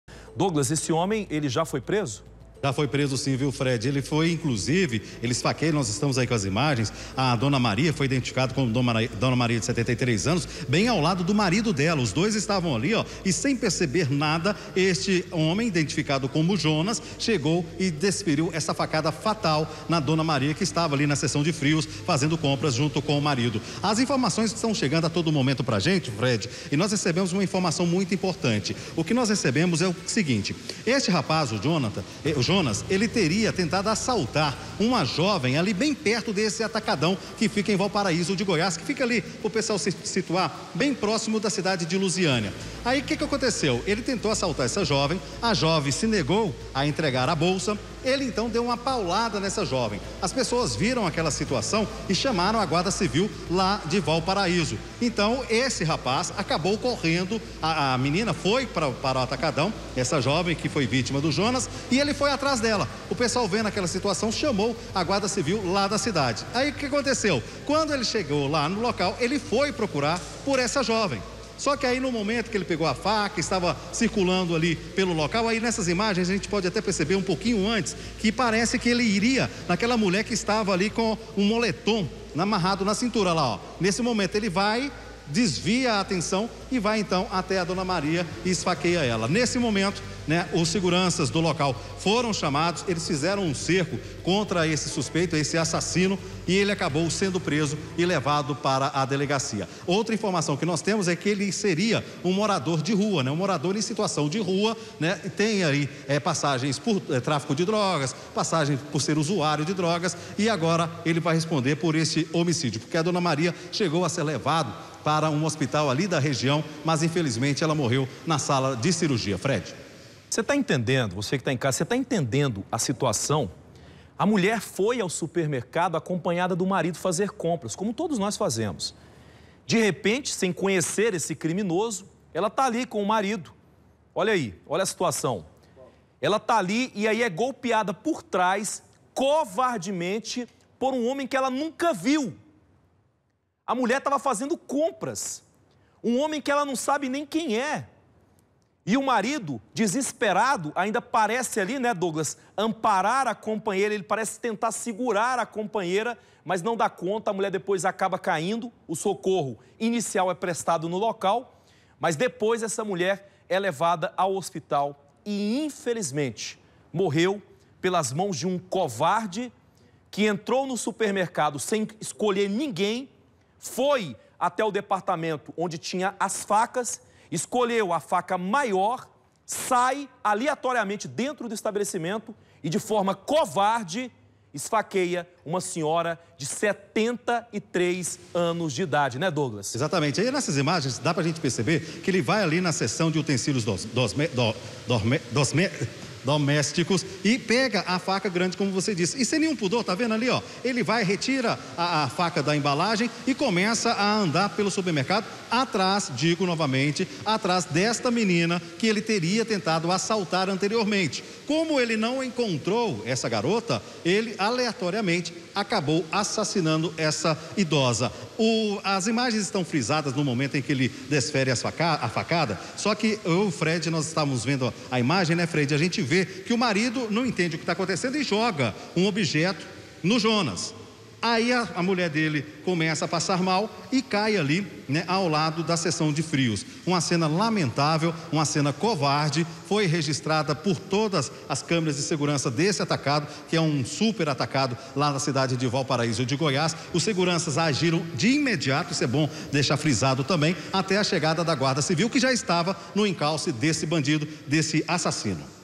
SEM EXPLICAÇÃO: HOMEM ESFAQUEIA IDOSA DENTRO DE SUPERMERCADO
UMA IDOSA DE 73 ANOS, QUE ESTAVA FAZENDO COMPRAS COM MARIDO. FOI ESFAQUEADA NO MEIO DO MERCADO. UMA CRUELDADE SEM TAMANHO. A VÍTIMA ESTAVA DE COSTAS QUANDO FOI ATACADA.